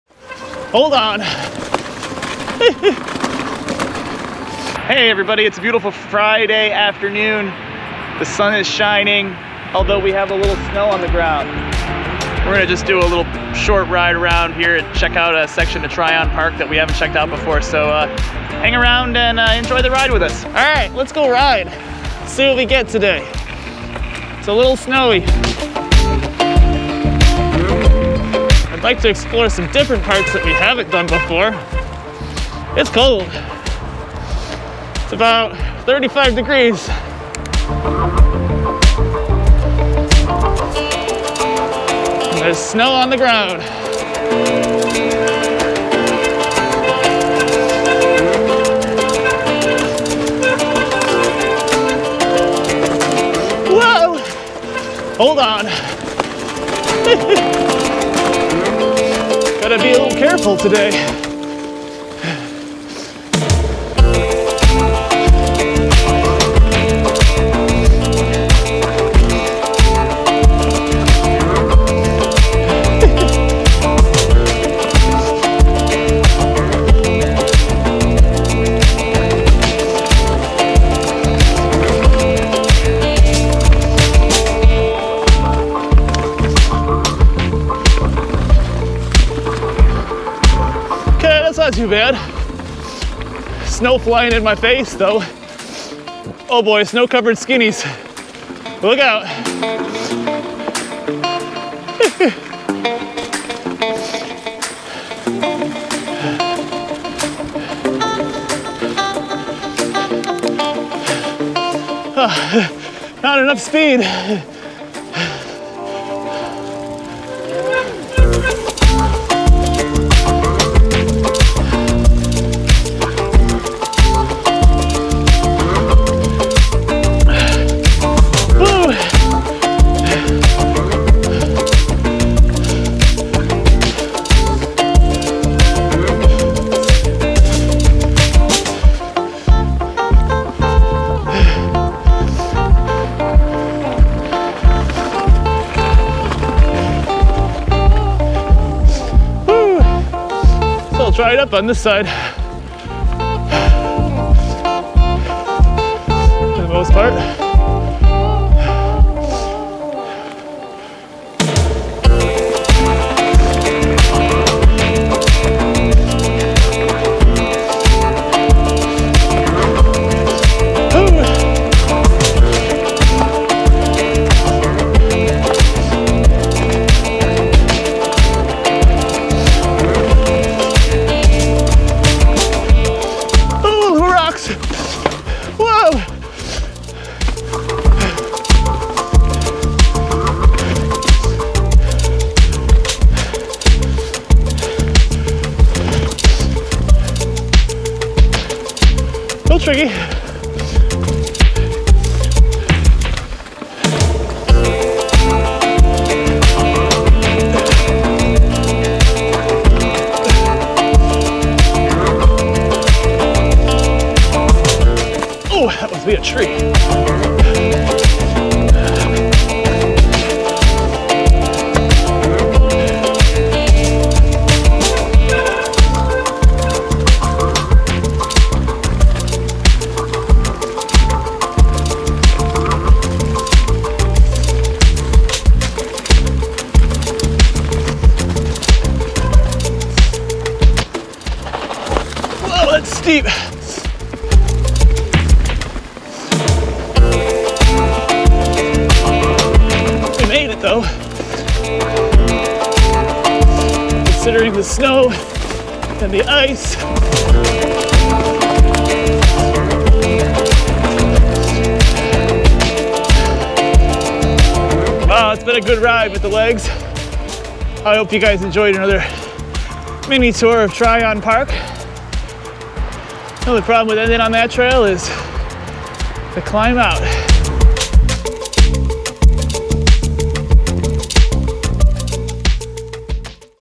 Wet snow, bike tires. Is this a good idea?
Fall has been taken over by winter temperatures and SNOW on the ground. Check out a little more mini tour of Tryon Park in Rochester, NY.

Follow our little snippets of fun over on Instagram as well